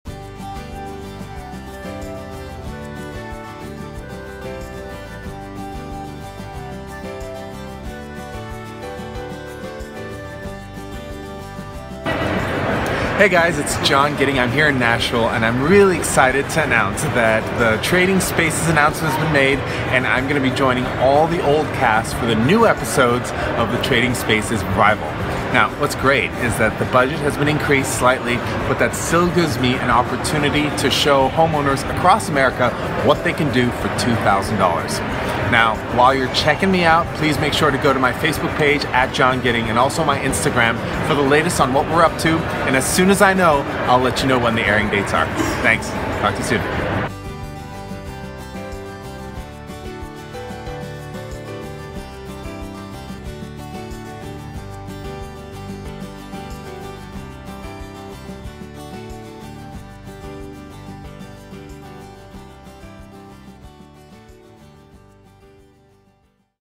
Trading Spaces Reboot - New Cast Member John Gidding
At the 2017 Nashville Home Show, John Gidding, former HGTV Curb Appeal host, shared his fun news that he'll be joining the new cast of TLC's Trading Spaces!  Be sure to check John out on Facebook & Instagram @johngidding He's a design expert who, with his experience in residential, commercial and landscape architecture as well as urban and graphic design, transforms average homes into sculptural showpieces. He has also holds a Yale undergrad degree & masters in architecture from Harvard!